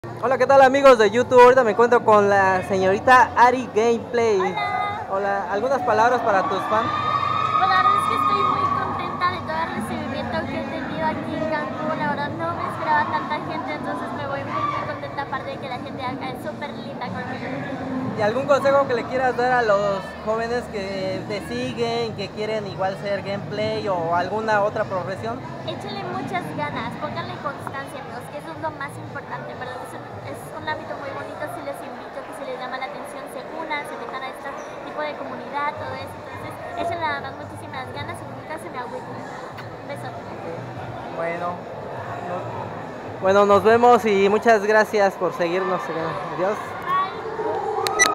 ARI GAMEPLAYS EN EXPO COMICS CANCUN 2019
HOLA AMIGOS DE YOUTUBE ENE ESTE PEQUEÑO VIDEO PUDISMOS ENTREVISTAR ALA SRTA ARI GAMEPLAYS EN LA EXPO COMIC CANCUN DONDE ENVIA ALGUNASO TIPS Y UN SALUDOS,